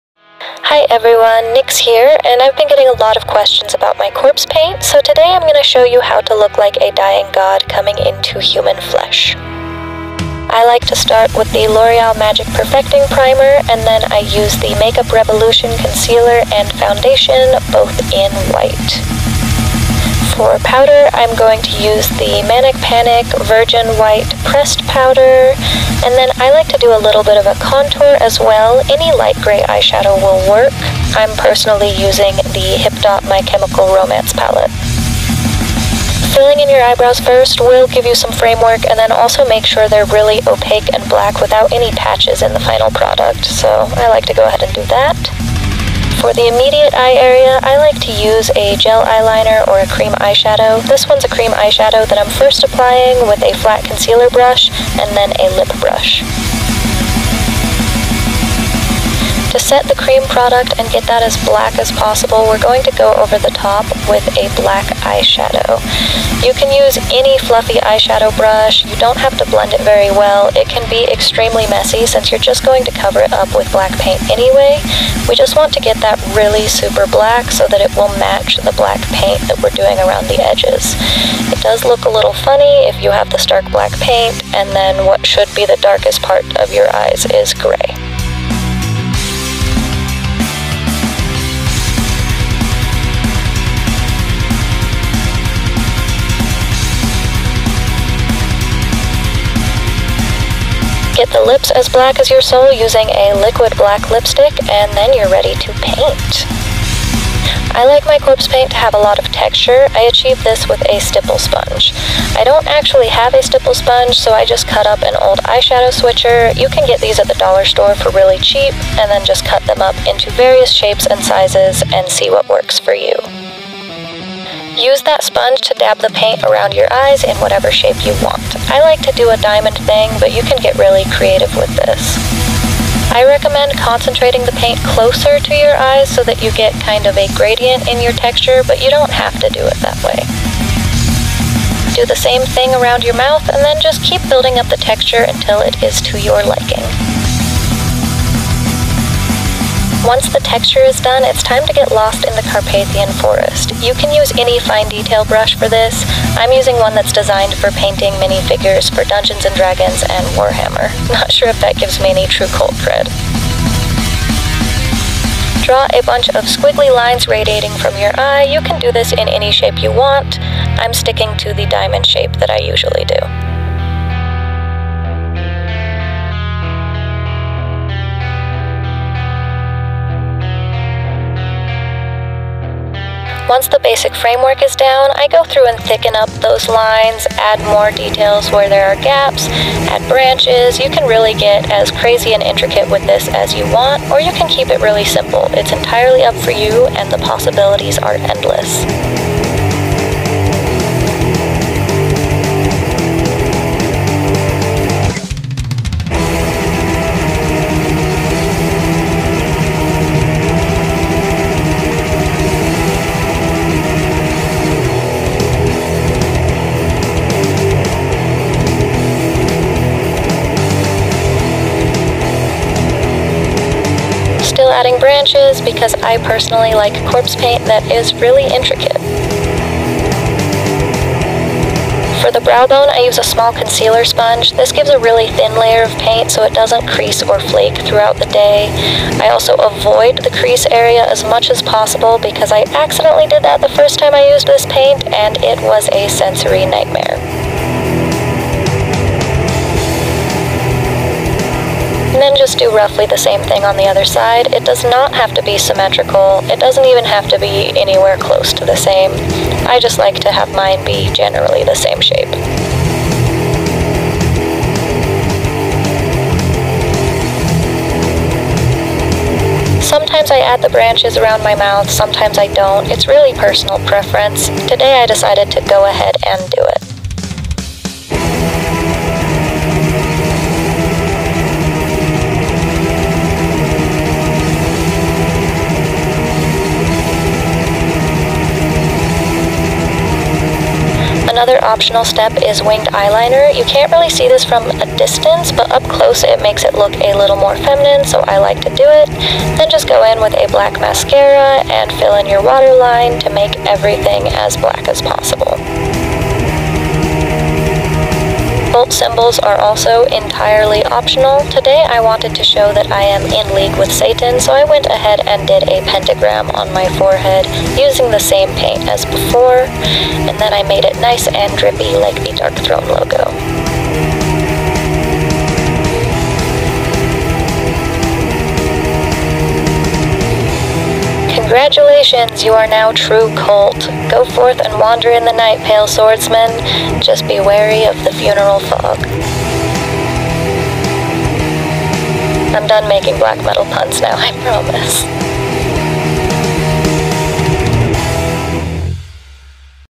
Black Metal Corpse Paint Tutorial
I've been getting a lot of questions about my black metal corpse paint, so I decided to go ahead and a make a makeup tutorial! Here's a list of the products I used:

L'Oreal -  Magic Perfecting Base
Makeup Revolution - Conceal & Correct Concealer (White)
Makeup Revolution - Conceal & Define Foundation (White)
Manic Panic - Virgin White Pressed Powder
Hipdot x MCR - Three Cheers Palette (Shotgun Sinners)
NYX Cosmetics - Dip, Shape, Go! (Black)
Maybelline - Color Tattoo (Risk Maker)
Sugarpill - Pressed Eyeshadow (Bulletproof)
NYX Cosmetics - Liquid Suede (Alien)
Mehron - Liquid Makeup (Black)
Physicians Formula - Eye Booster Waterproof Ultra-Fine Liquid Eyeliner (Blackest Black)
Maybelline - Great Lash (Blackest Black)
Urban Decay - 24/7 Waterline (Legend)

I hope you found this video informative and helpful, go forth and Ơ̷̡̡̟͔̜̳̥̙̱̼̼̲̹͇̟̔̍̈̑̇̽̌͗ͅÚ̸̟͇̄̂̀͐͆̅̔͗̋̓͘̕͜͠͝G̴̡̢̨͕̘͕̙̓̀͛͂̎͘͘H̸͙͍͚͈͈̳͈̳͉̬̀̃̒͘